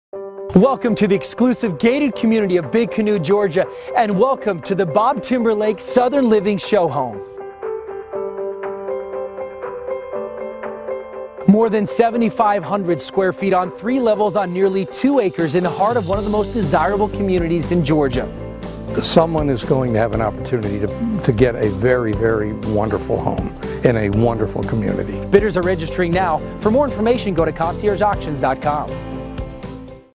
Look Video // Tranquility Knoll // Big Canoe, GA
Set on over 1.7+ acres inside the serene mountain resort community of Big Canoe is an arts and crafts masterpiece like no other. Completely customized by world-renown American artist and designer Bob Timberlake as a Southern Living Show Home, Tranquility Knoll is the true definition of one-of-a-kind. Like a finely-faceted jewel, this residence was crafted with impeccable detail — every door, faucet, and chandelier was custom designed and fabricated to spec for this special residence.

Life here is a dream come true. A perfectly-placed estate — soft southeastern winds and glorious sunrises, set at 3,000± foot elevation with clear mountain views, and complete privacy from neighbors — in an award-winning community with amenities galore. Enjoy daily strolls, hikes or bike rides through the more than 20+ miles of lush paths and paved trails. Take a dip at the swim club or in Lake Disharoon. Visit the marina to gear up for a day of fishing on Lake Petit. There’s never a shortage of activities or scenic enclaves in which to relax and enjoy the surrounding natural forested preserve.